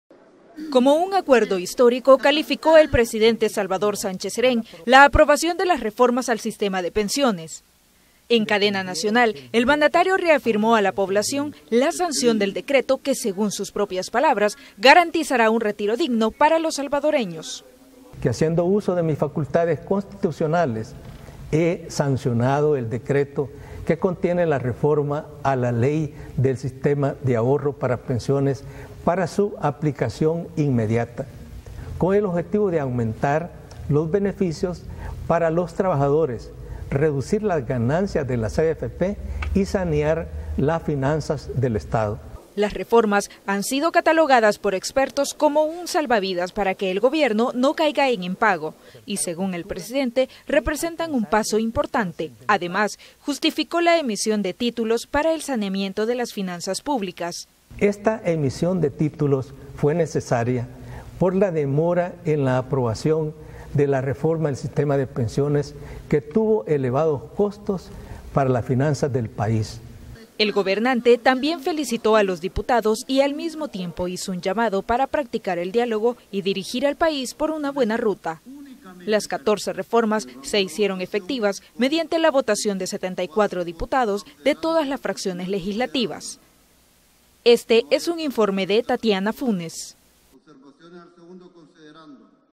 CN: Presidente agradeció la aprobación de la reforma de pensiones
El Presidente de la República en cadena nacional, agradeció a los diputados en la Asamblea Legislativa y a todos los que aportaron con sus propuestas para hacer las modificaciones, el mandatario reconoció que el sistema deberá ser evaluado de forma permanente.